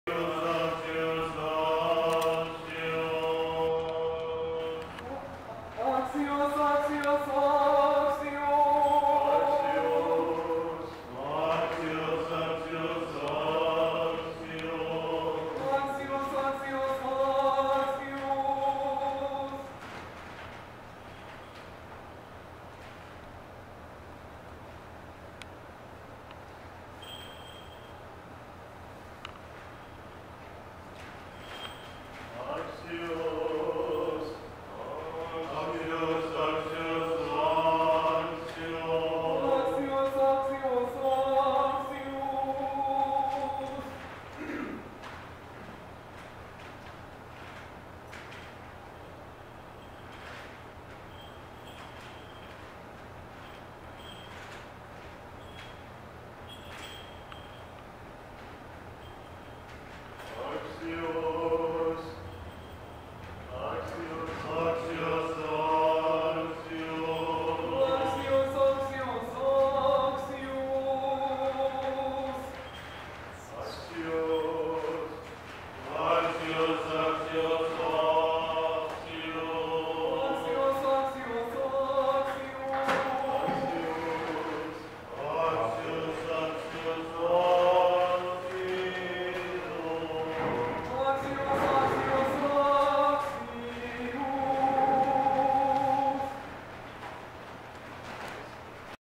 Ordination to the Priesthood
Ordination of Fr. Gabriel Ridenour to the Holy Priesthood by Metropolitan Hilarion of the Russian Orthodox Church Outside of Russia (6/22/17).